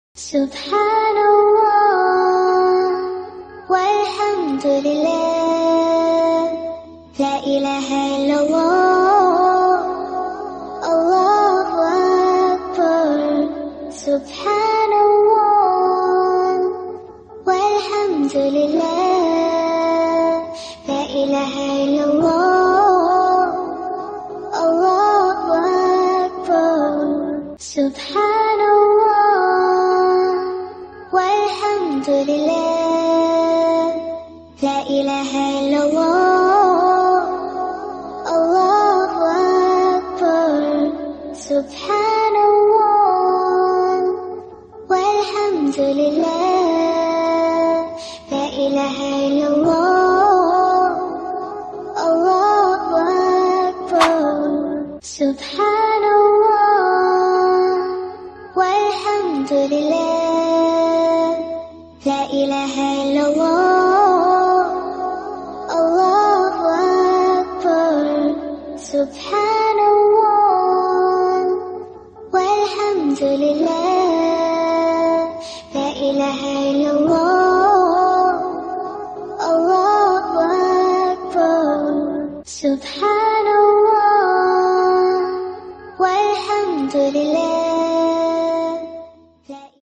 How to write Allah in Arabic calligraphy | Easy tutorial for beginners | ALLAH Name Calligraphy
In this video I will share my practice of Arabic Calligraphy with different colors. Write name of Allah in Sulus Font / Thuluth Font